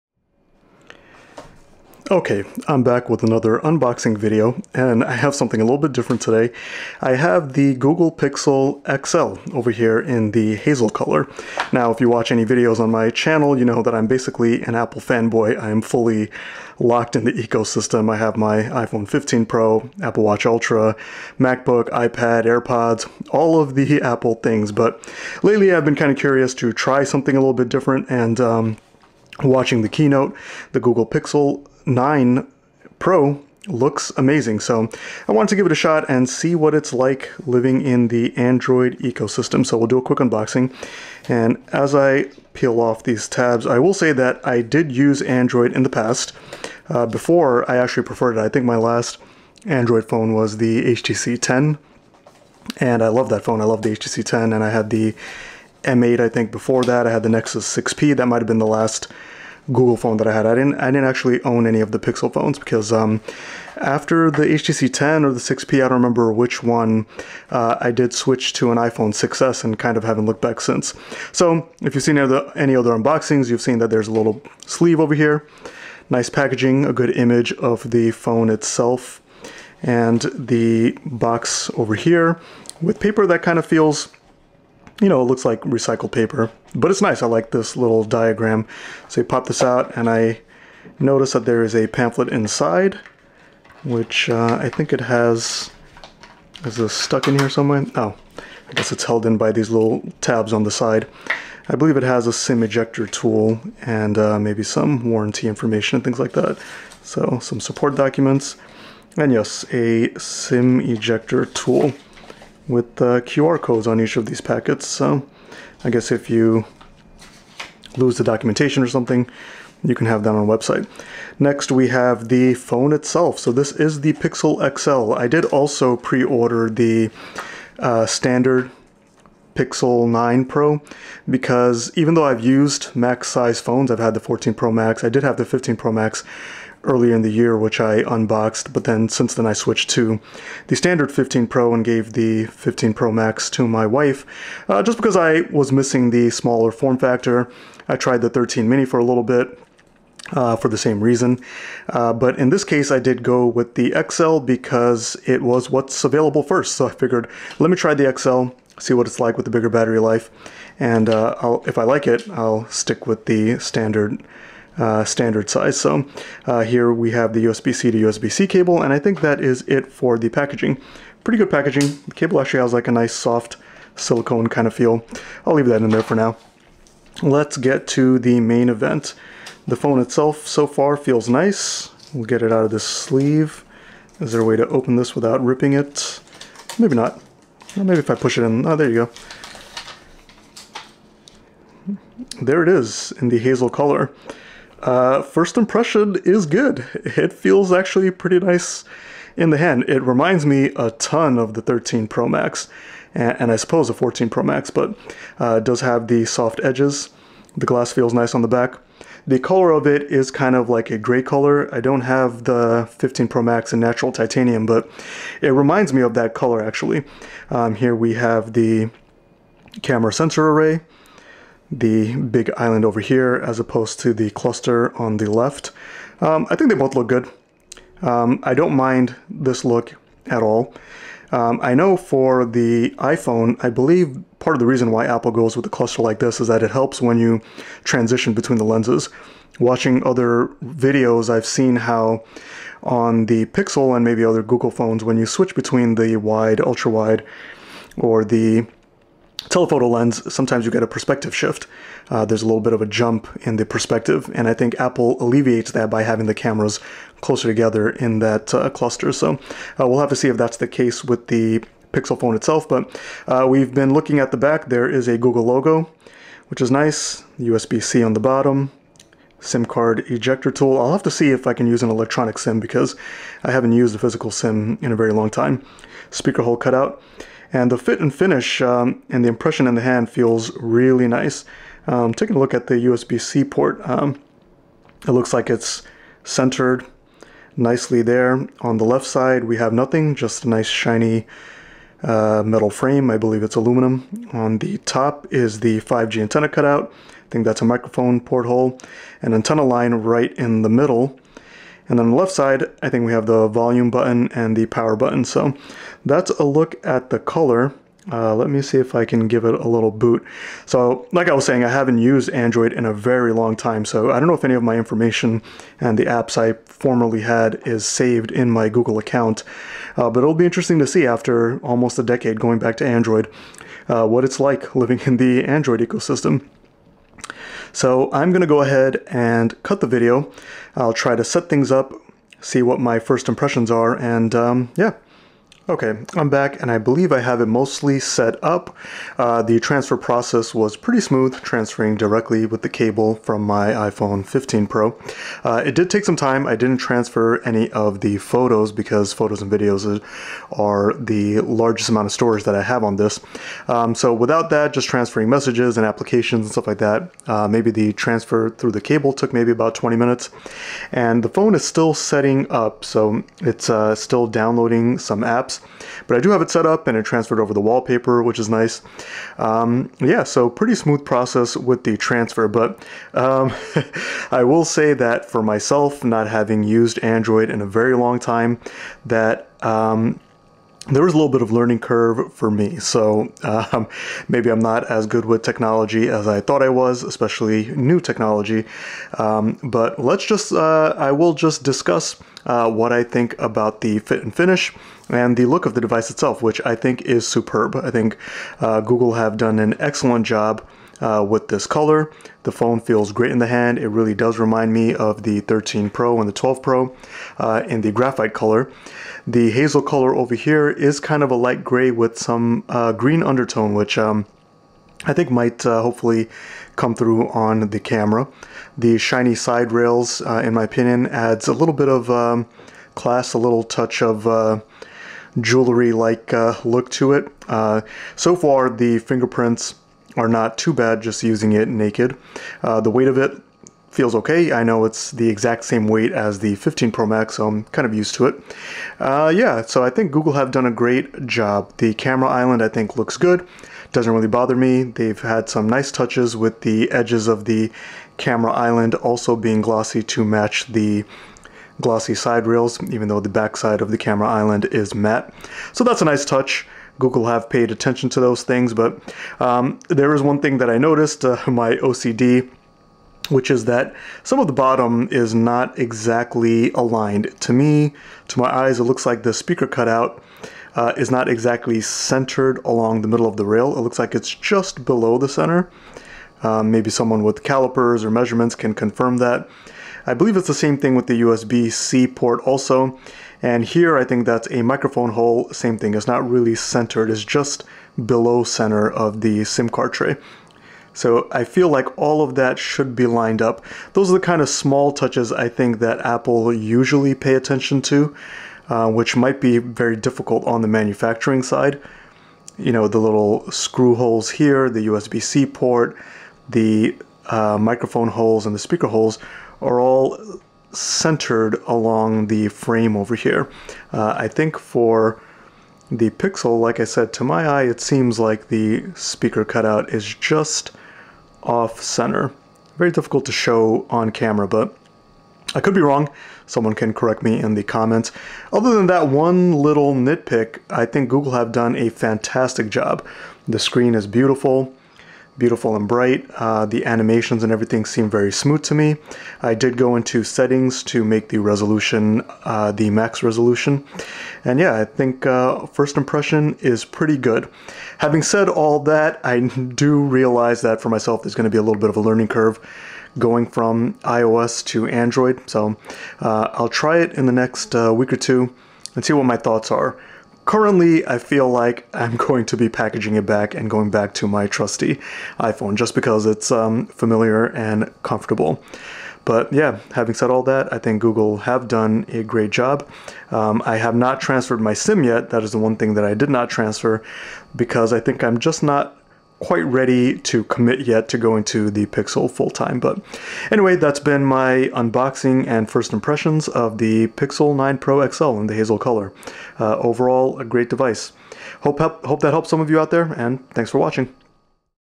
Google Pixel 9 Pro XL Unboxing and First Impressions: from a long time iPhone User
Unboxing and first hands on look at the Google Pixel 9 XL Pro in the hazel color from an Apple fanboy.

Definitely a well made and good looking device, but switching to Android after a decade of iOS will be a challenge